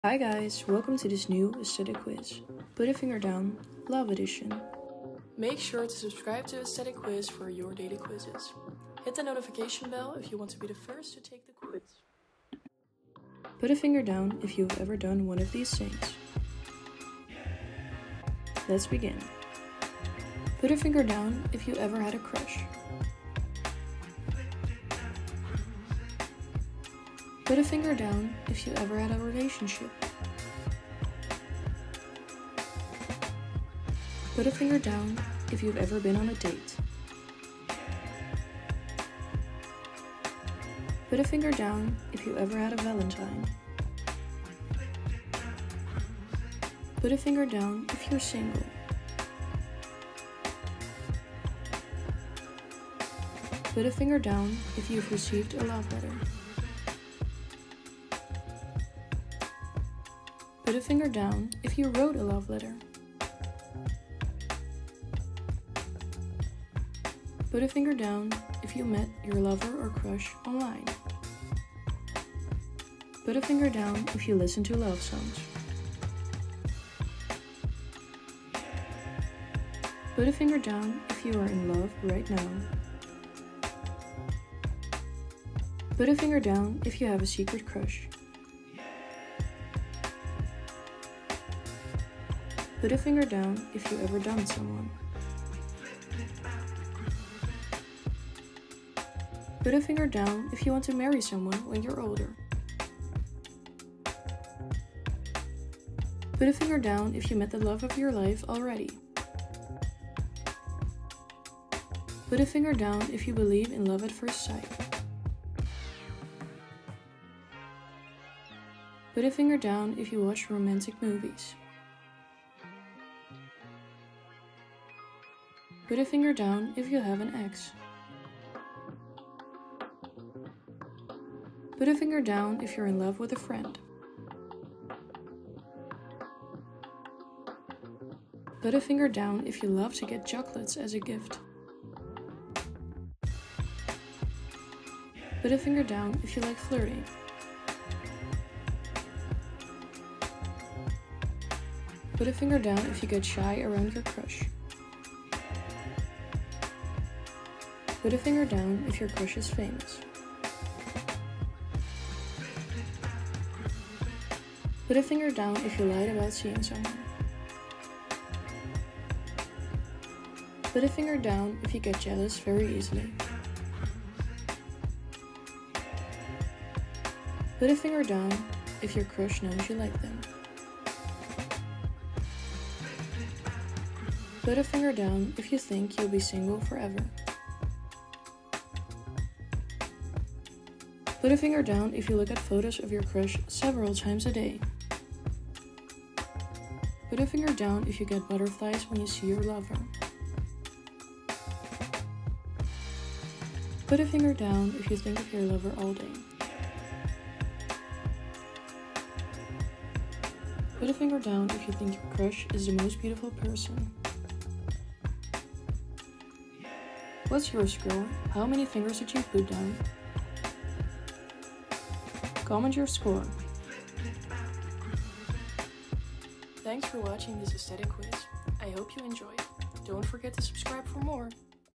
🥰PUT A FINGER DOWN: LOVE EDITION🥰 - Aesthetic Quiz
This is a put a finger down LOVE edition! How many fingers did you put down? Who is your true love?

About Aesthetic Facts
🎥 QUIZZES & entertainment

On this channel you’ll find fun and educational quizzes.

🟢 General knowledge quiz: coming soon!